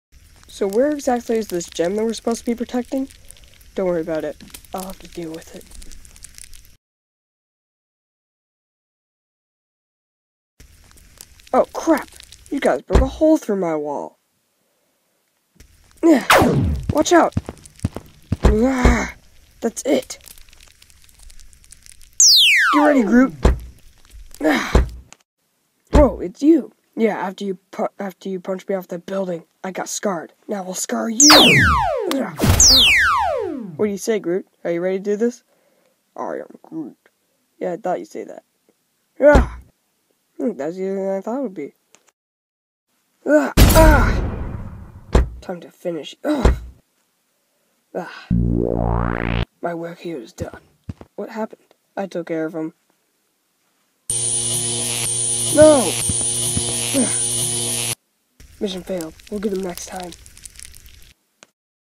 Infinity War Ep.4
Doctor Strange and his team fight against Mordo and Yellow Jacket to protect the time stone.